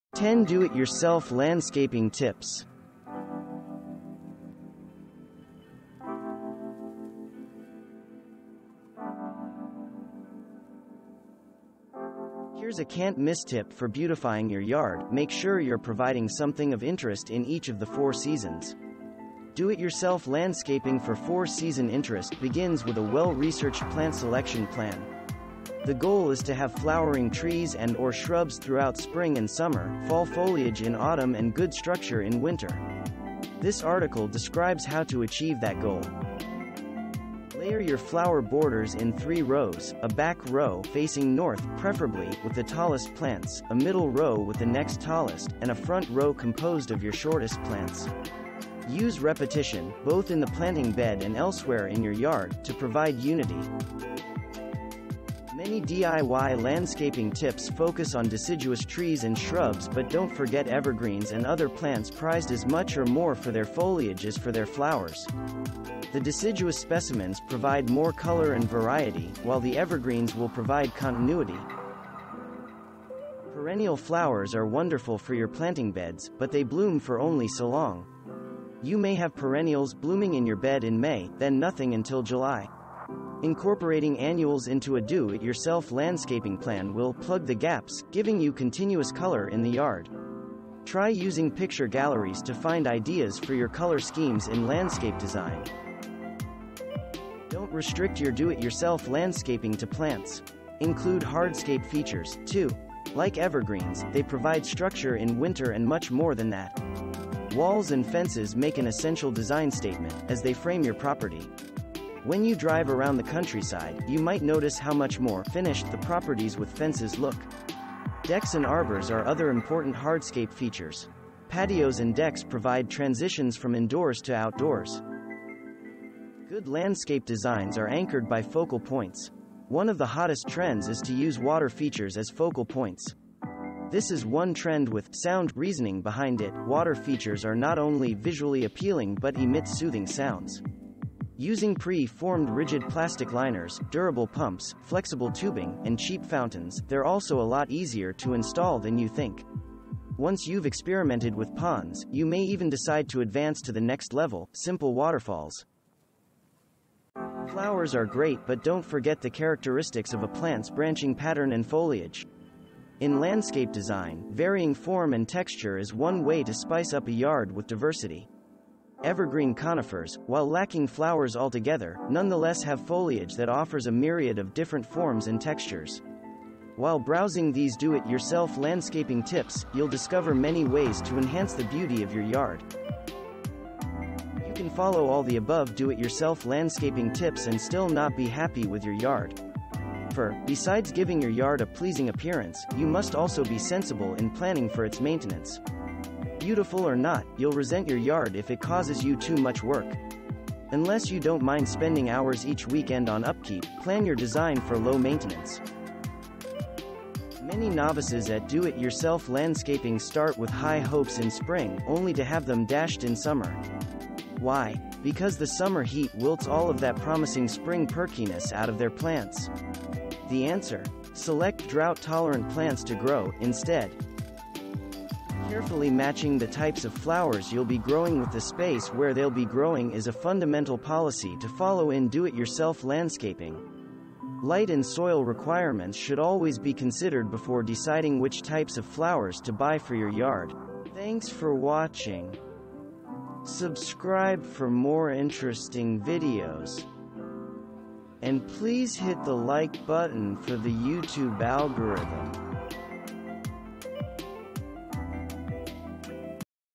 10 Do-It-Yourself Landscaping Tips 🛋️
Home landscaping can be challenging. Read these do-it-yourself landscaping tips and get ideas for how to make your yard look like a professional job.
0:00 - Intro
0:00:12 -   Year-Round Interest
0:00:21 -   Layer Your Flower Beds
0:00:30 -   Creating Continuity
0:00:39 -   Annuals Supplement Perennial Color
0:00:48 -   Incorporate Hardscape
0:00:57 -   Install Water Features: It's Easier Than You Think
0:01:06 -   Using Form and Texture
0:01:15 -   Make Your Life Easier With a Low-Maintenance Yard
0:01:24 -   Use Drought-Resistant Plants
0:01:33 -   Types of Flowers